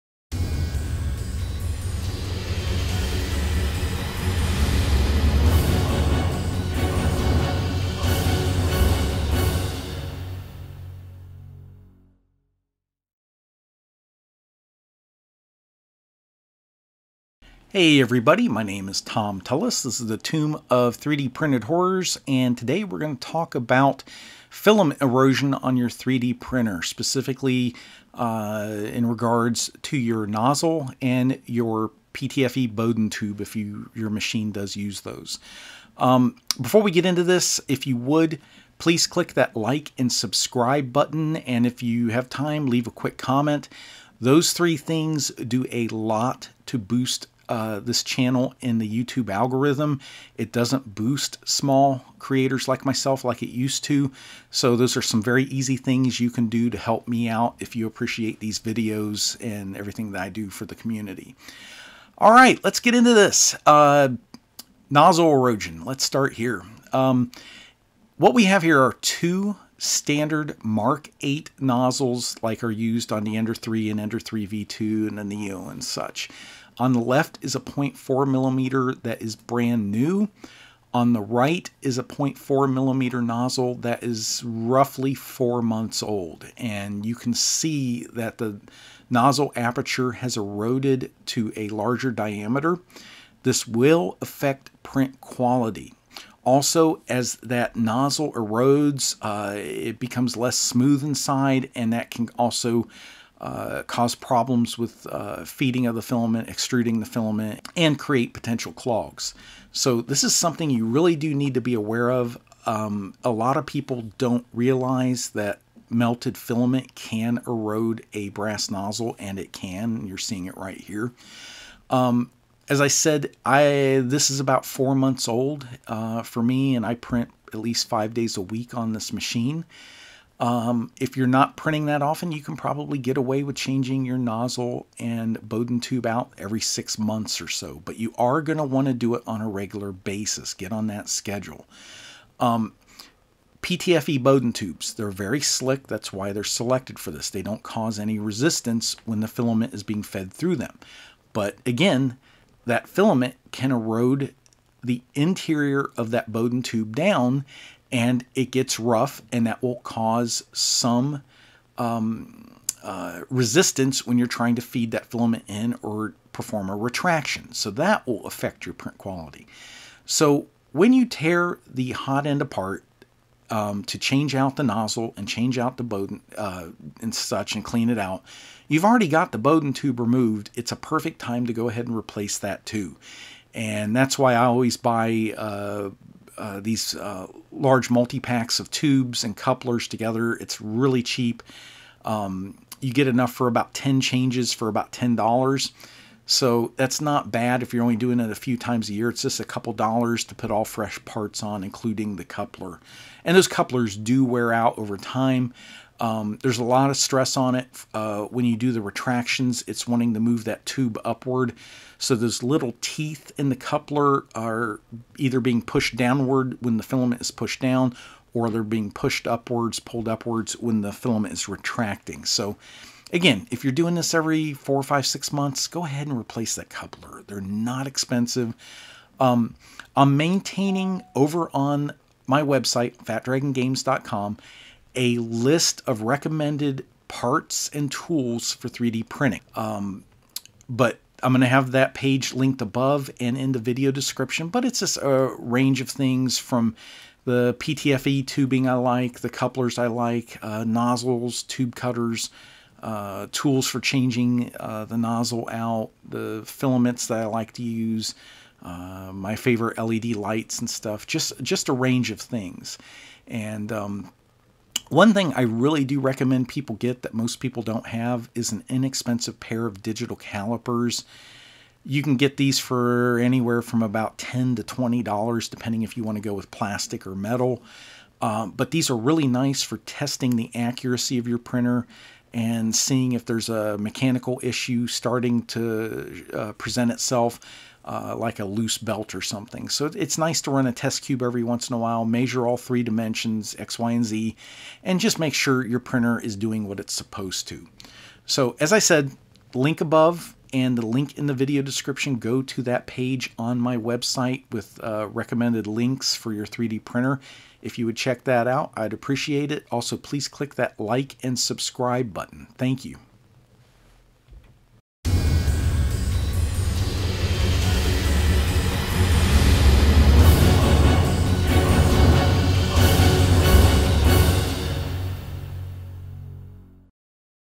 NOZZLE EROSION! What to look for and when to replace them!
I'll show the effects of filament erosion on nozzle aperture diameter and recommended timetable for replacing. PARTS

For help printing our Dragonlock and Crypt Classics models join our Facebook group